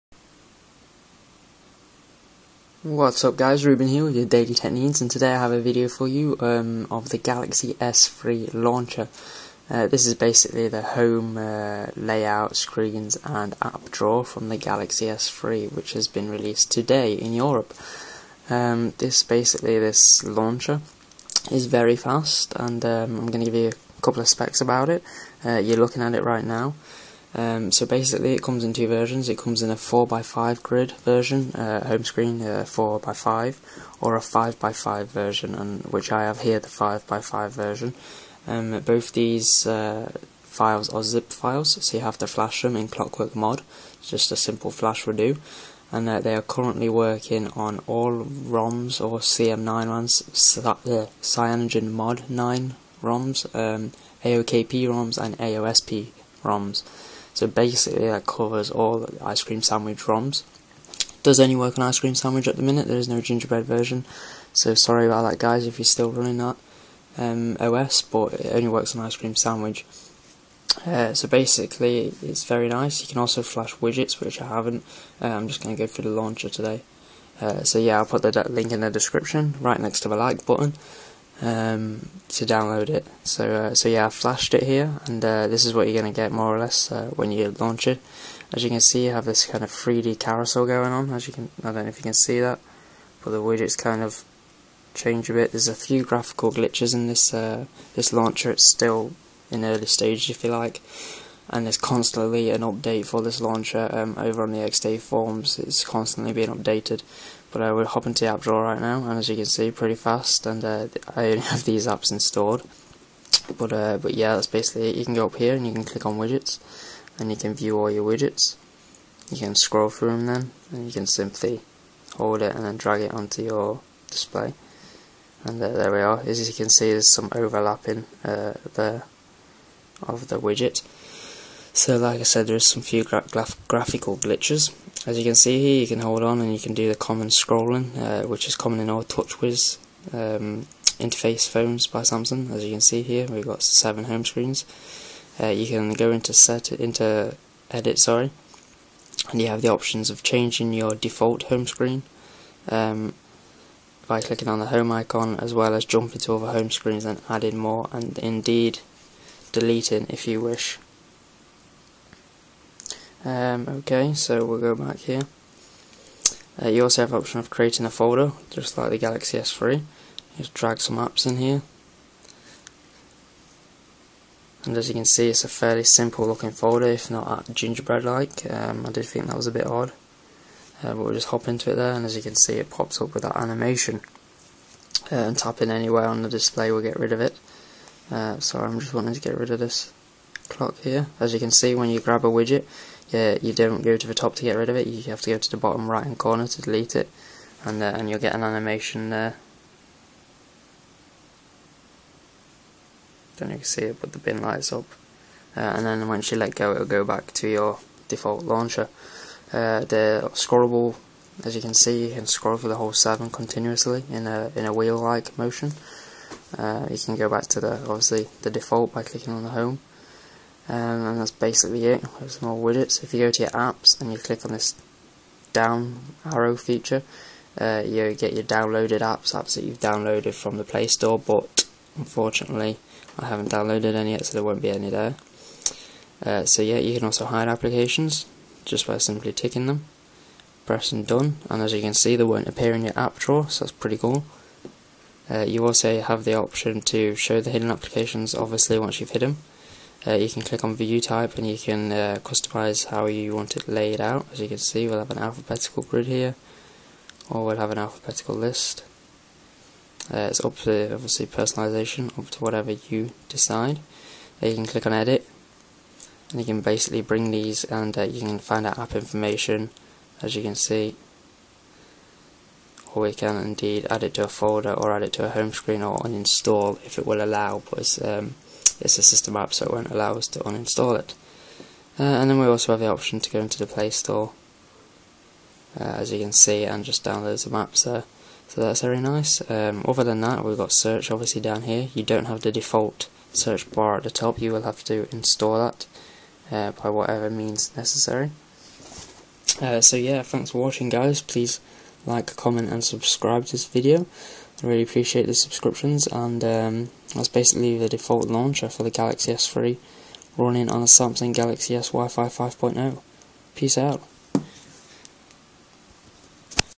Samsung Galaxy S3 Launcher on any Android device
Hi guys today I will take a quick look at the Galaxy S3 Launcher available on XDA for most ICS 4.0 customs ROOMS.

Requirements:

Rooted
ICS ROM
Certain DPI e.g. 240 view thread below: